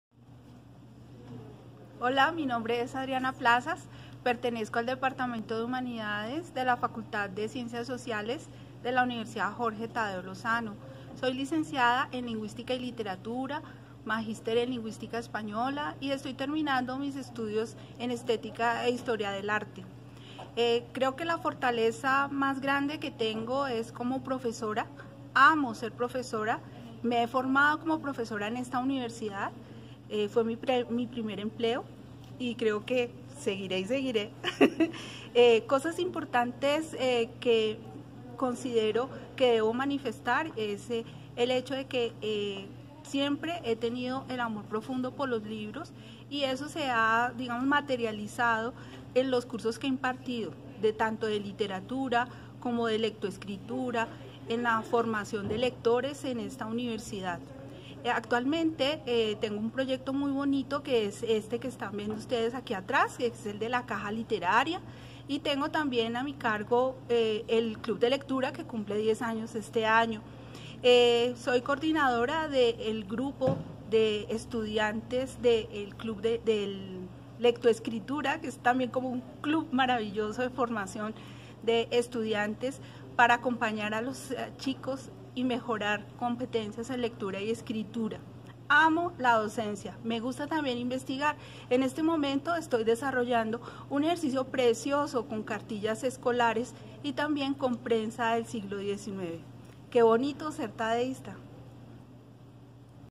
Adriana Margarita Plazas-Salamanca
Magister en estudios de lingüística española, se ha dedicado a reflexionar en los fenómenos de la comunicación en general y en las formas de comunicación entre los jóvenes en particular. Ha realizado investigación en la enseñanza de la lectoescritura en la población universitaria. Actualmente dirije el proyecto de lectoescritura de la Universidad Jorge Tadeo Lozano el cual busca el mejoramiento de las competencias comunicativas de los estudiantes que inician los estudios superiores. Profesora de literatura en la Universidad de Bogotá Jorge Tadeo Lozano. Directora del club de lectura Tadeísta. Coordinadora de eventos literarios en la Biblioteca de la Tadeo.

Maestría en Lingüística Española. Seminario “Andrés Bello” Instituto Caro y Cuervo.

Maestría en Estética e Historia del Arte. Universidad Jorge Tadeo lozano. En curso.